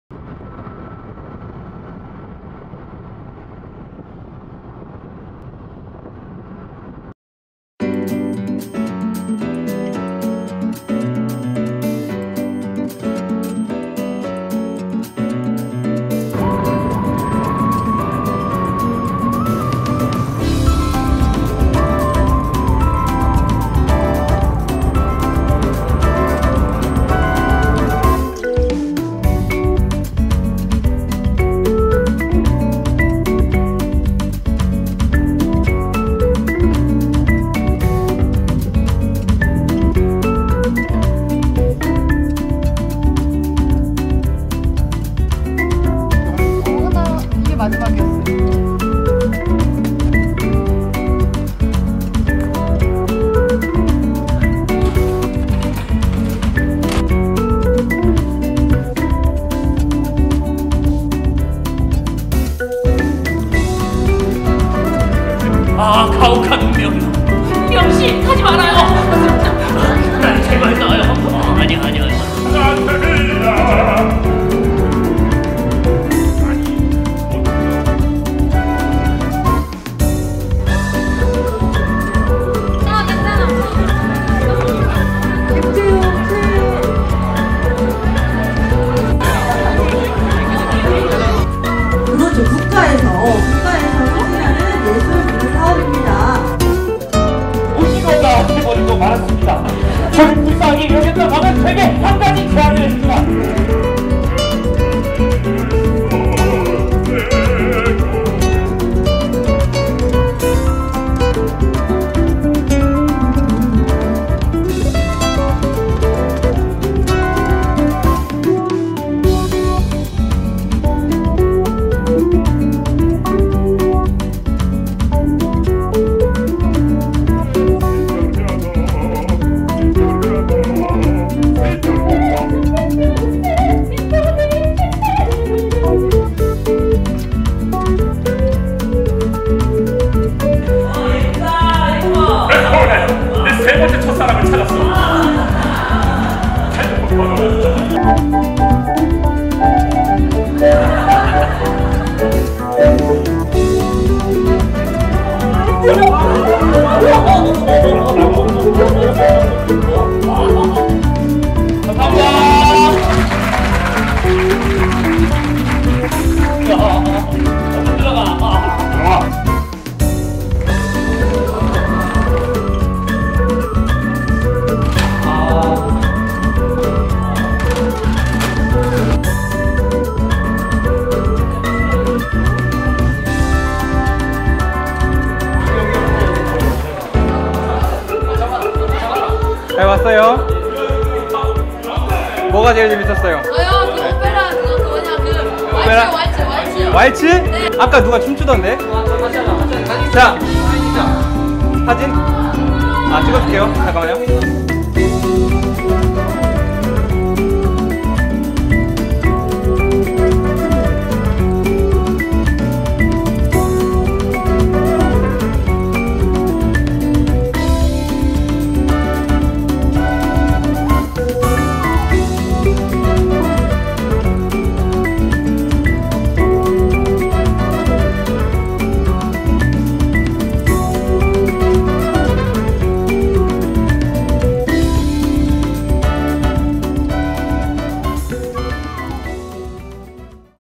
2016 신나는 예술여행 - 우리 학교 하우스콘서트 (하반기)
2016 신나는 예술여행 - 우리 학교 하우스콘서트

연주 : 앙상블 그리오, 오페라팩토리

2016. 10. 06 광양 광영중학교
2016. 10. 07 하동 악양중학교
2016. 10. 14 평택 진위중고등학교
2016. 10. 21 하동 청암중학교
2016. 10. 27 안성 만정중학교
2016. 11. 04 정선 함백중학교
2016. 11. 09 해남 해남고등학교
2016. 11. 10 순창 동계고등학교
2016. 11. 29 화성 두레자연고등학교

주최 : 한국문화예술위원회
주관 : 더하우스콘서트
후원 : 문화체육관광부, 복권위원회
협력 : 교육부